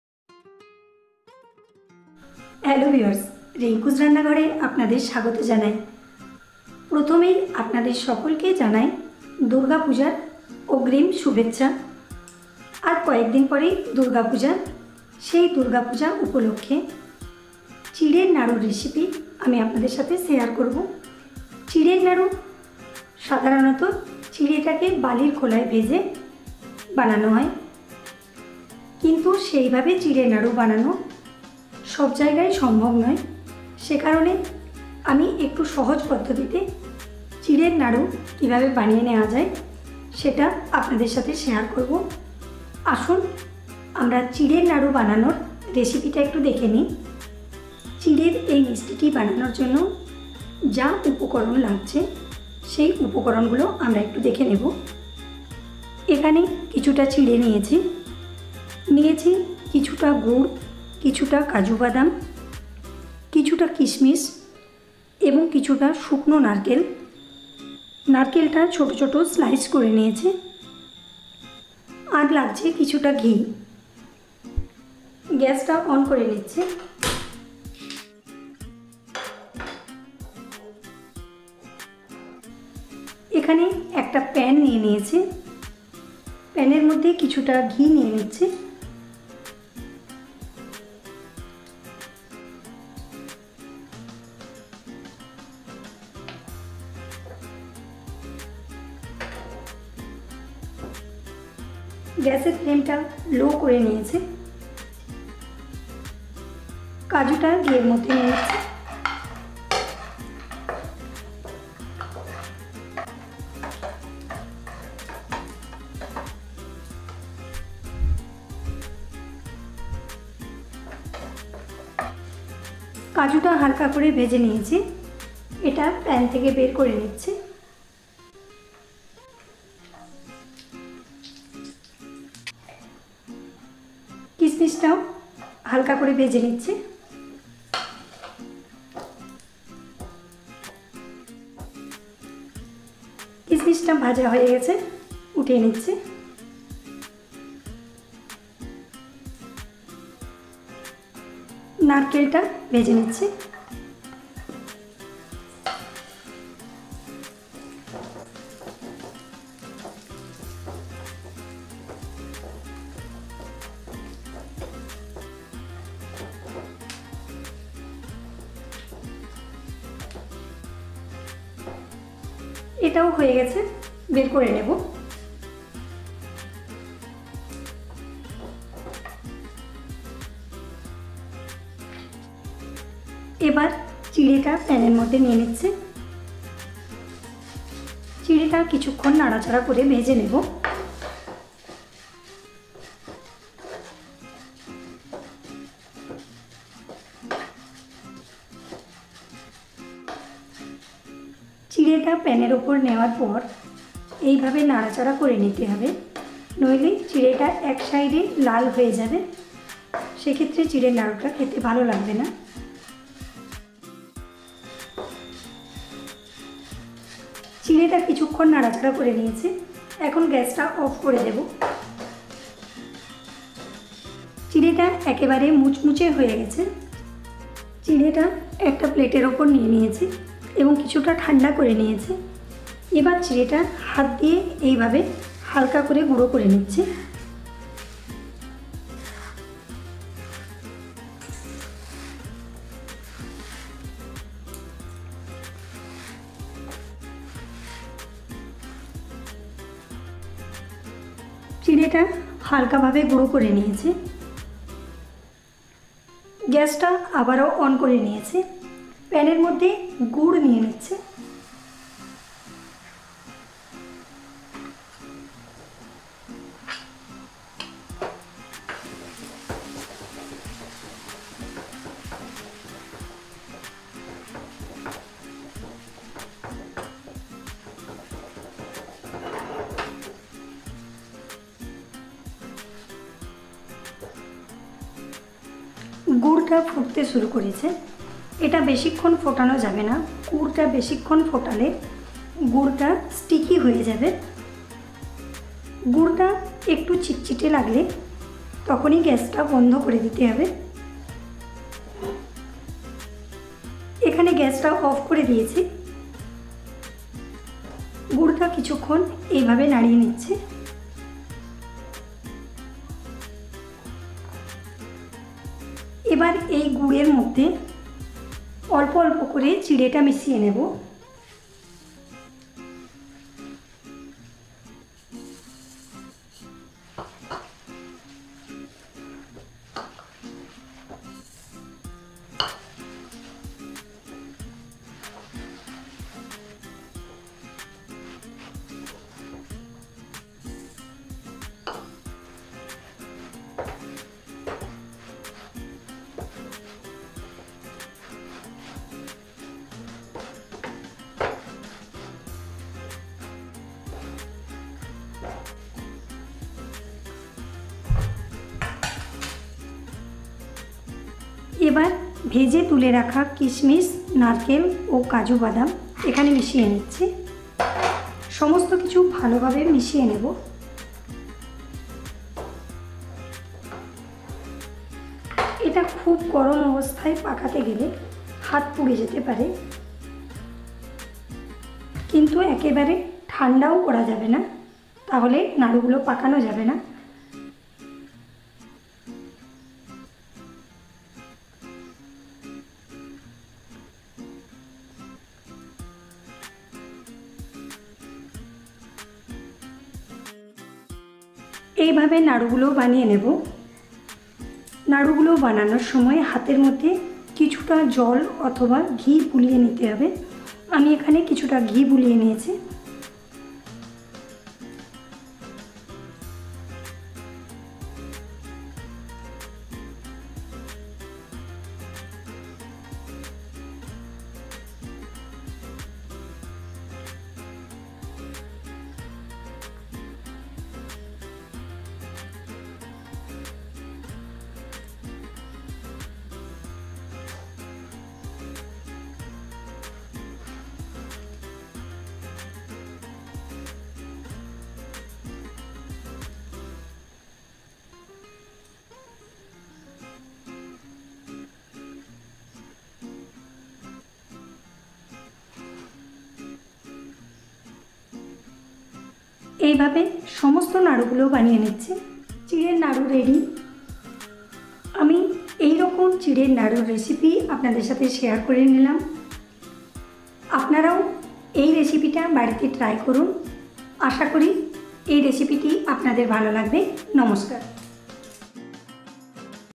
পুজা স্পেশাল চিঁড়ের নাড়ু রেসিপি | Chirer Naru Recipe | Poha Gud Ladoo
A very essensial ladoo of all of the Bengali's in the pujo times.

Ingredients :-

Beaten rice/ Chire/ Poha (চিঁড়ে)
ghee (ঘী)
Jaggery / Gur / Gud (গুড়)